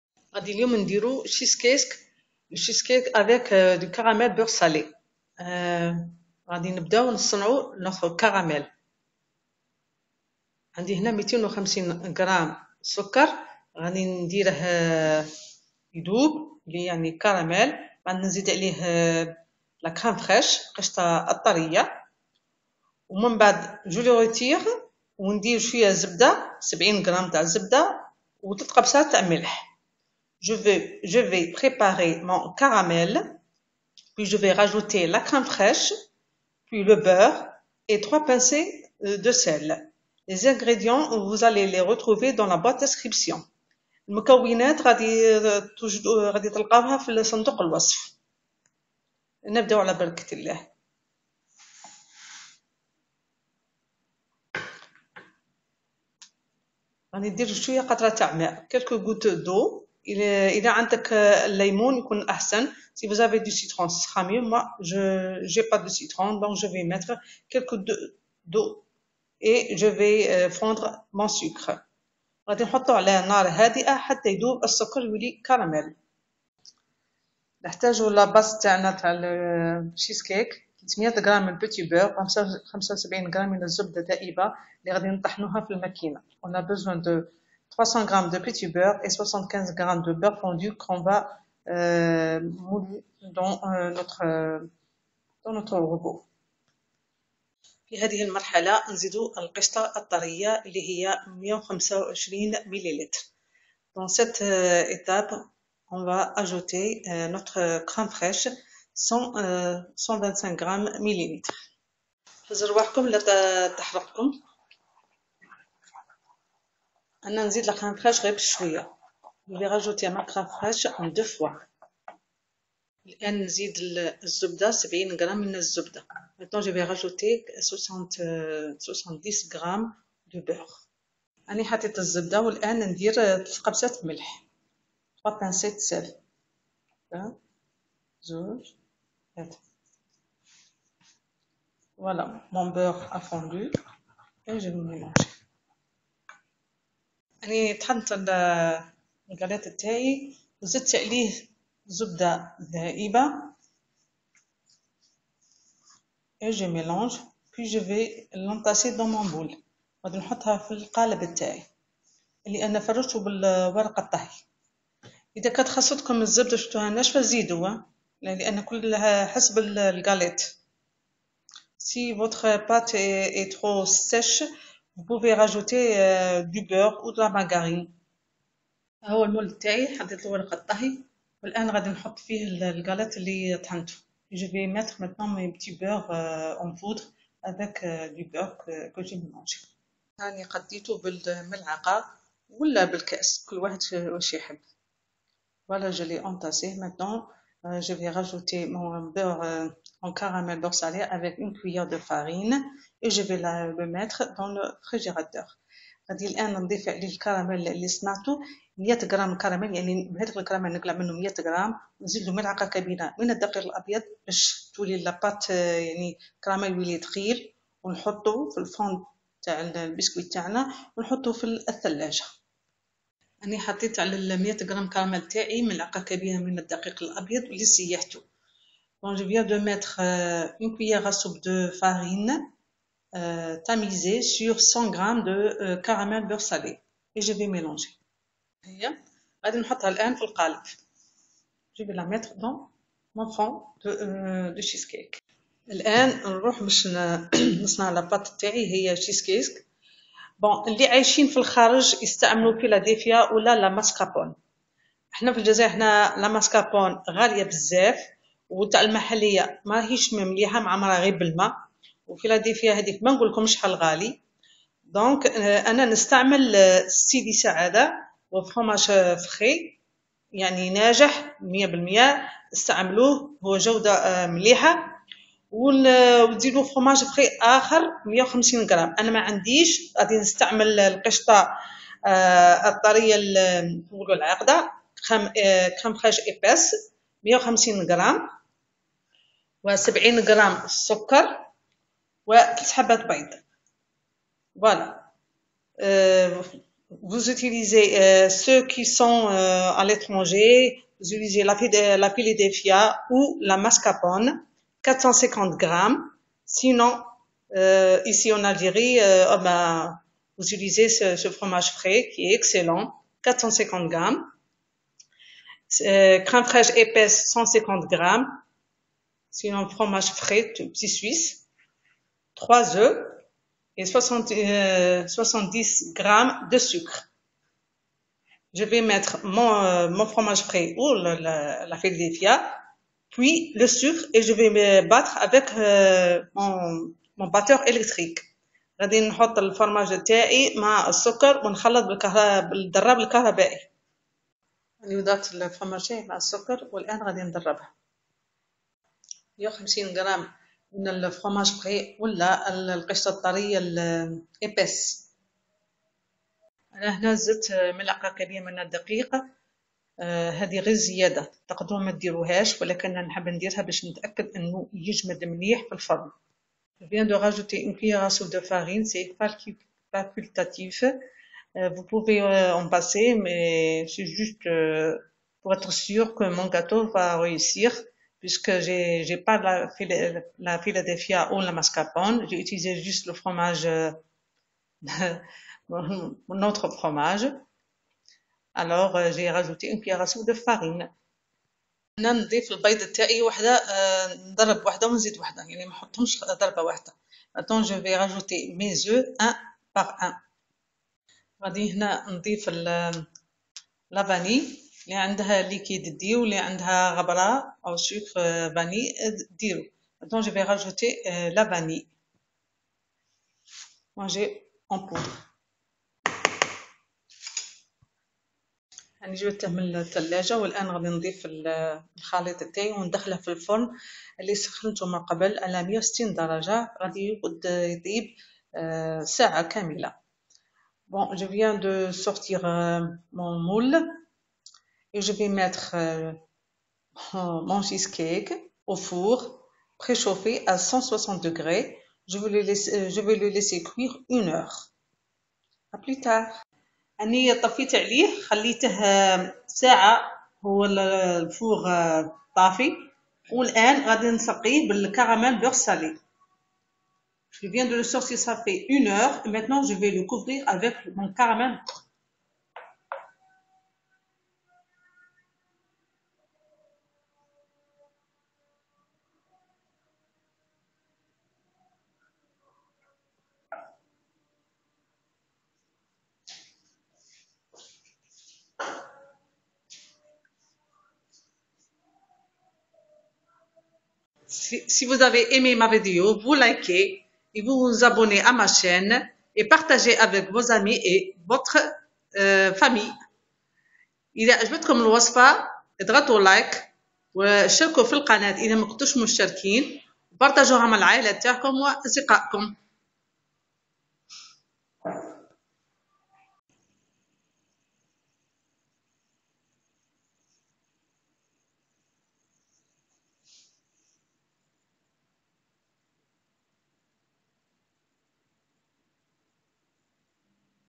Gâteau au Fromage et au Caramel - شيز كيك بالكراميل يا سلام
INGRÉDIENTS POUR UN CHEESECAKE CARAMEL DE 10 PERSONNES :
Caramel au beurre salé :
• 250 g de sucre en poudre
• Quelques gouttes d'eau
• 125 ml de crème liquide
• 3 pincées de fleur de sel
• 70 g de beurre demi-sel
Réserver 100 g
• 30 g de farine
Base biscuitée :
• 300 g de petit biscuit au beurre
• 75 g de beurre
Garniture :
• 450 g de fromage frais
• 70 g de sucre en poudre
• 3 œufs
• 150 g de crème fraîche épaisse
• 1 cuillère à café de poudre vanille
1 h 10 à 160°C
Laisser refroidir dans le four

INGREDIENTS FOR A CHEESECAKE CARAMEL OF 10 PEOPLE:
Salted butter caramel :
• 250 g caster sugar
• A few drops of water
• 125 ml of liquid cream
• 3 pinches of fleur de sel
• 70 g half-salted butter
Reserve 100 g
• 30 g flour
Biscuit base:
• 300 g small butter biscuit
• 75 g of butter
Garnish :
• 450 g fresh cheese
• 70 g caster sugar
• 3 eggs
• 150 g thick cream
• 1 teaspoon of vanilla powder
1 h 10 to 160 ° C
Cool in the oven